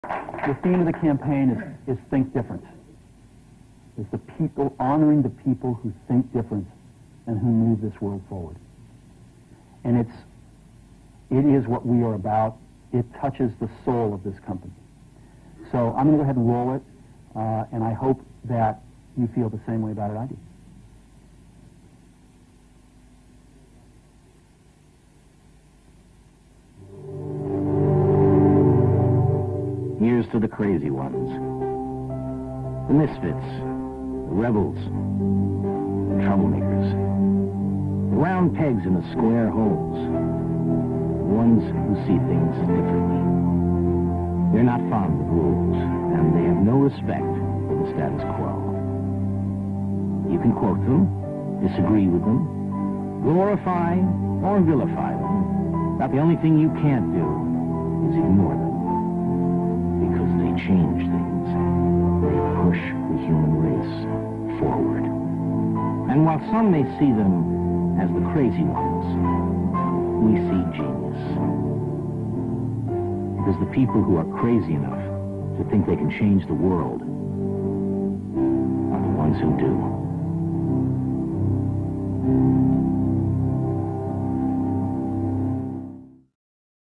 Apple - Here's to the Crazy Ones
Prefaced by Steve Jobs - Here's to the Crazy Ones. Cheers! Always will be inspired by Steve.